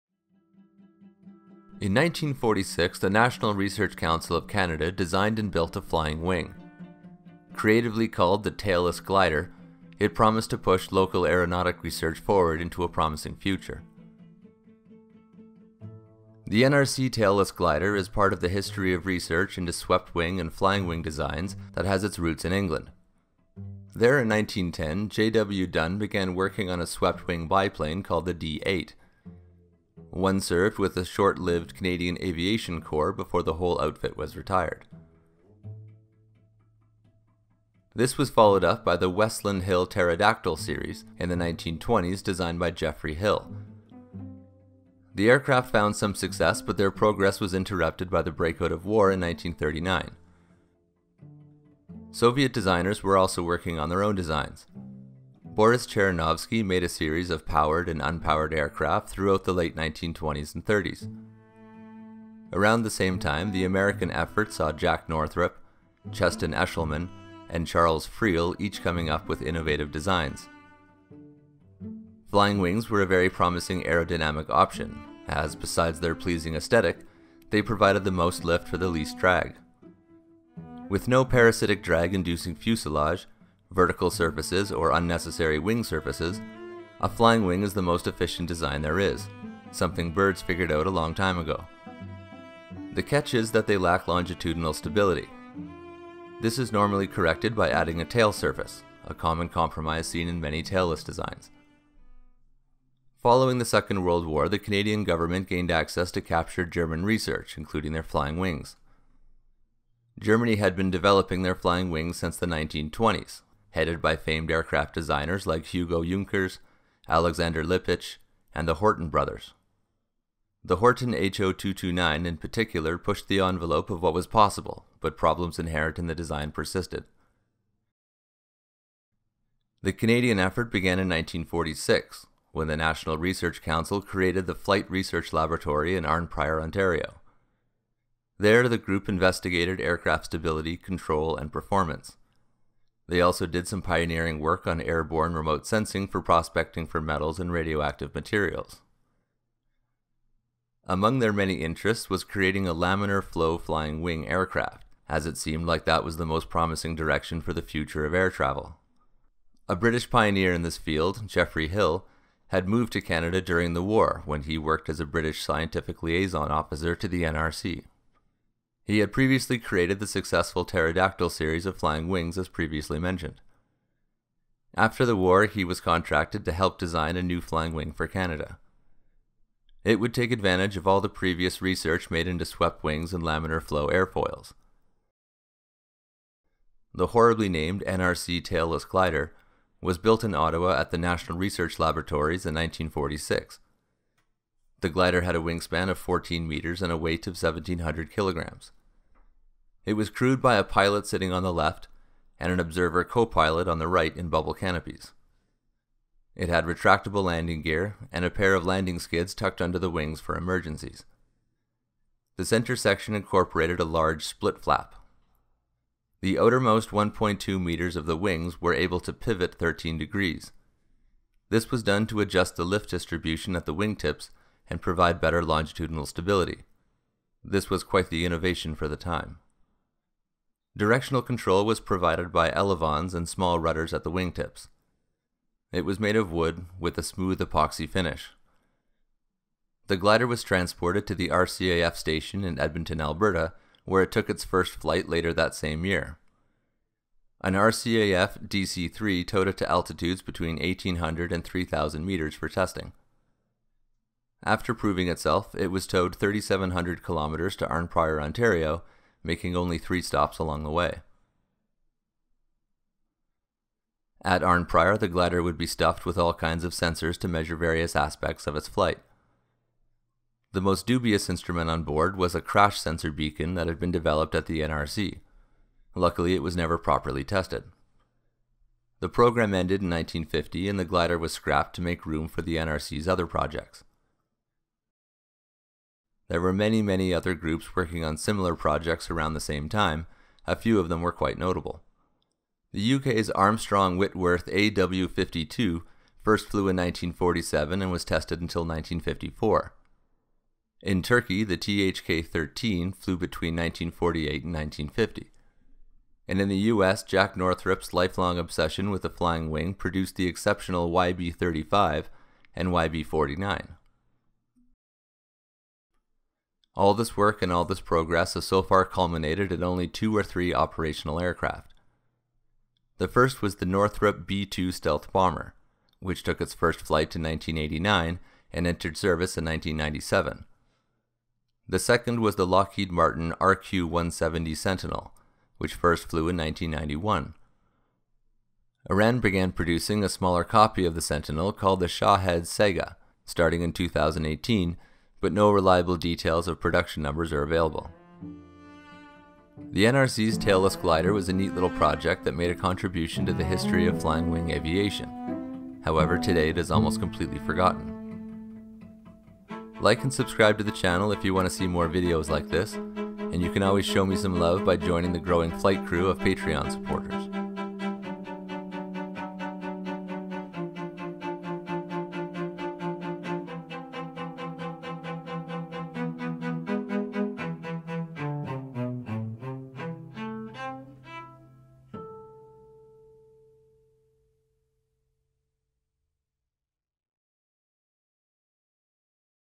Canada's Almost Forgotten Flying Wing; the NRC Tailless Glider
In 1946 the National Research Council of Canada designed and built a flying wing. Creatively called the Tailless Glider, it promised to push local aeronautic research forward into a promising future. It is part of the history of research into swept wing and flying wing designs that has its roots in England and proceeded through the next 110 years.

0:00 Introduction
0:15 Early Research
1:42 Captured Designs
2:08 Canadian Effort
4:45 Modern Designs
5:49 Conclusion